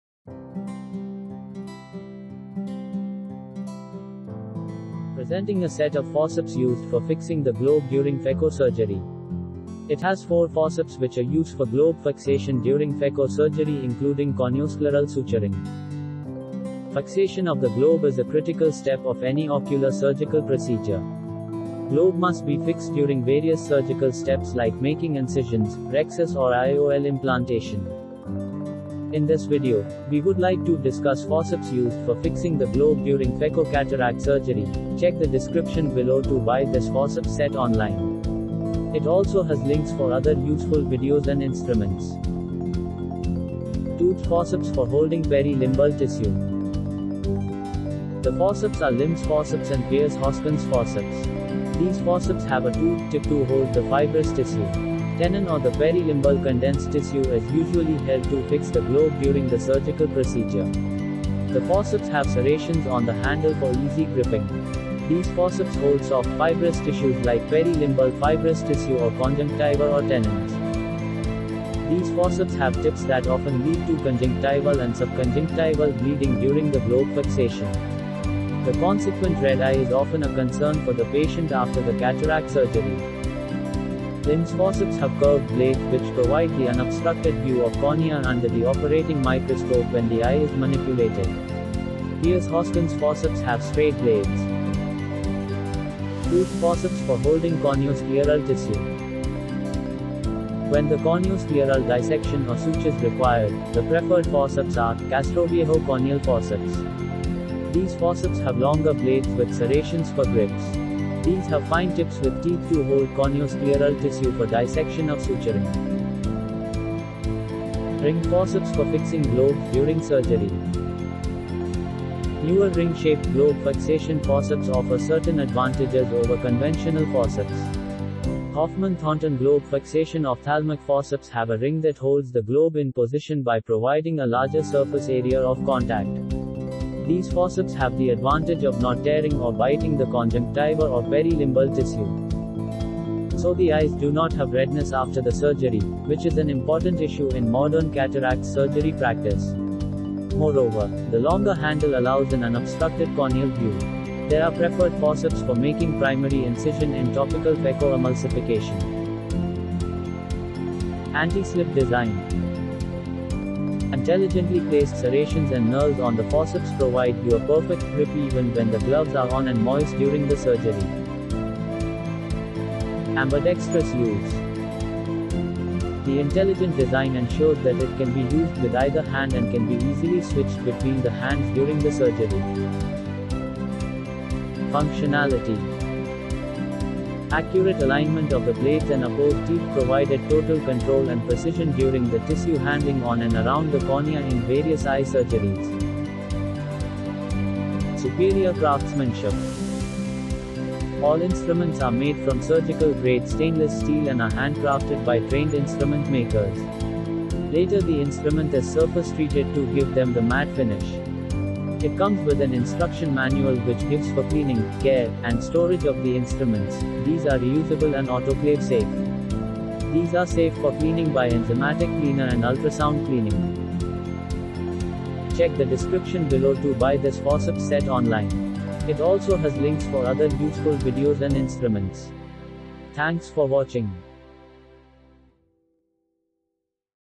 Globe Fixation Ophthalmic Forceps For Phaco Surgery
Fixation of the globe is a very critical step of any ocular surgical procedure. Globe has to be fixed during various surgical steps like making incisions, IOL implantation, or dissection.

Toothed forceps for holding peri-limbal tissue.
The forceps are Lim's forceps and Pierce Hoskins Forceps.

Toothed Forceps For Holding Corneo-Scleral Tissue.
 The preferred forceps are, Castroviejo corneal forceps

Ringed forceps for fixing globes during surgery.
Hoffman Thornton Globe Fixation Ophthalmic Forceps